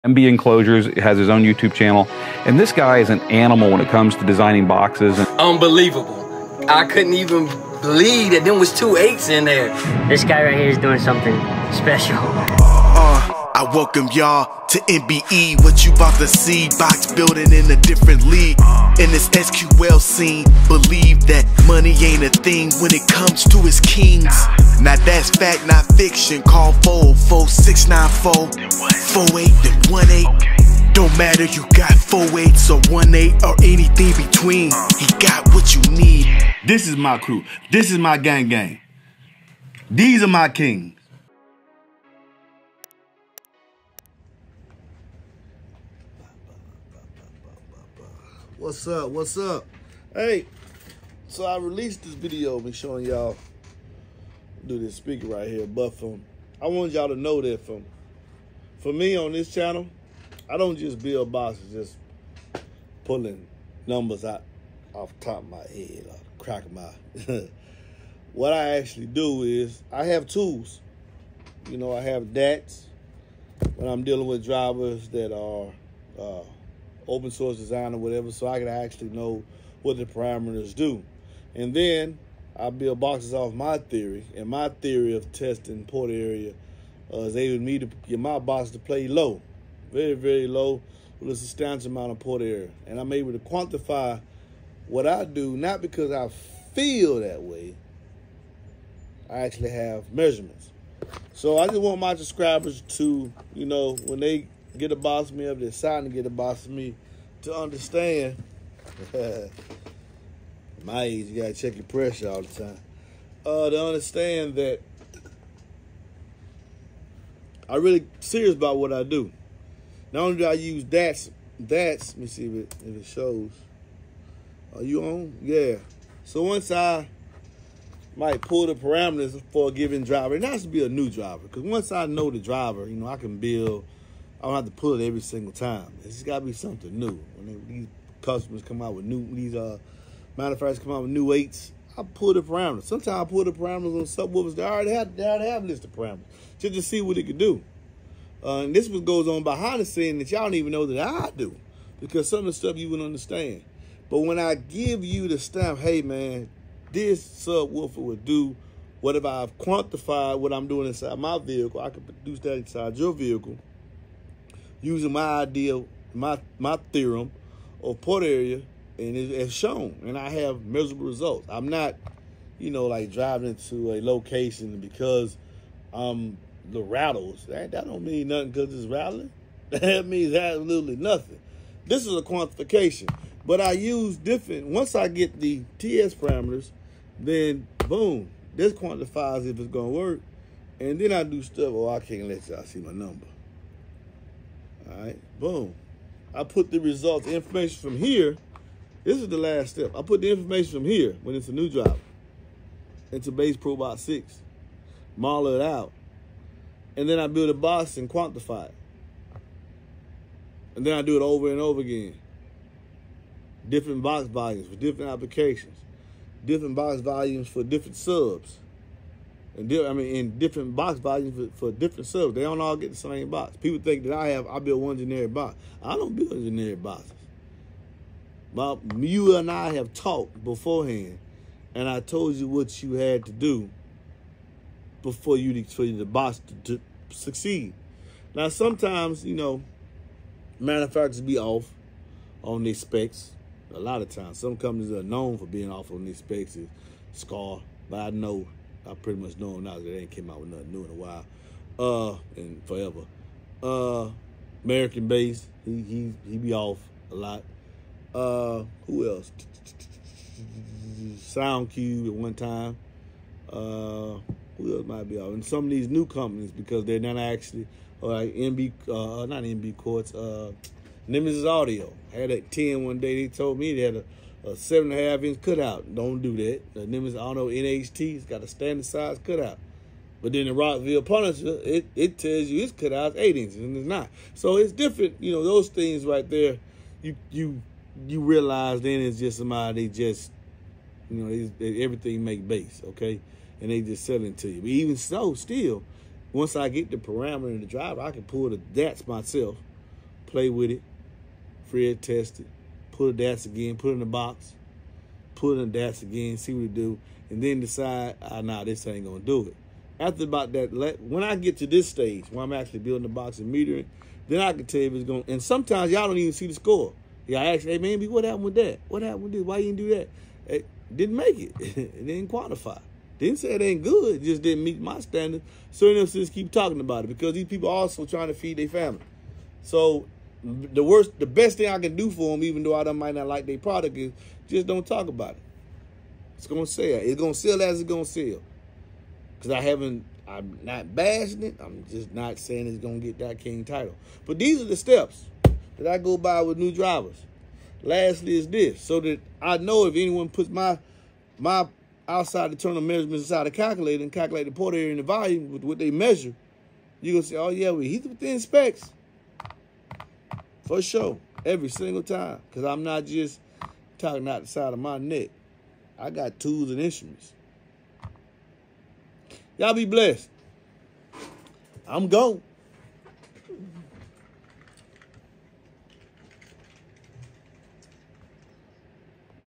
When and Why I Use Quantitative Tools For Subwoofer and Enclosure Testing!!!
To order an enclosure please call or text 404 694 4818